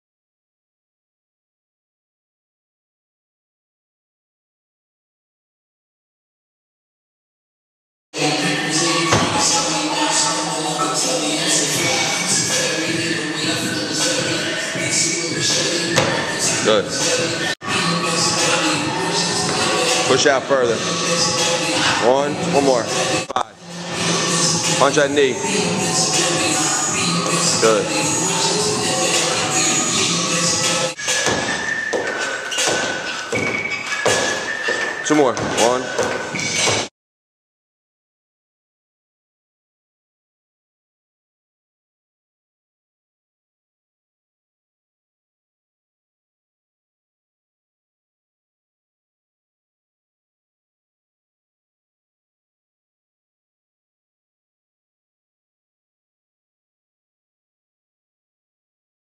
Band Plyo Jump Series
This is a short series of Plyos using bands.   This is a more advanced series of Jumps to provide extra resistance & still allow for explosion needed.
1. Band Broad Jumps
2.  Band Lateral Bounds
3.  Banded Box Push Offs
4.  Banded Lateral Box Ickey Shuffles (lateral box step overs)
5.  Banded Lateral Jump Switches
Always stray on the looser side rather than starting with tight bands.  You can easily attach bands to each other to give some length in the bands to be able to attach it to a base.  Also, make sure when attaching the bands to a base that the base is very sturdy.  Sometimes, the force of a jump can easily knock over the base or pull it with the jump.  Use safety first attitude.  Always make sure that non resisted plyo work is mastered before moving on to any resisted plyo work.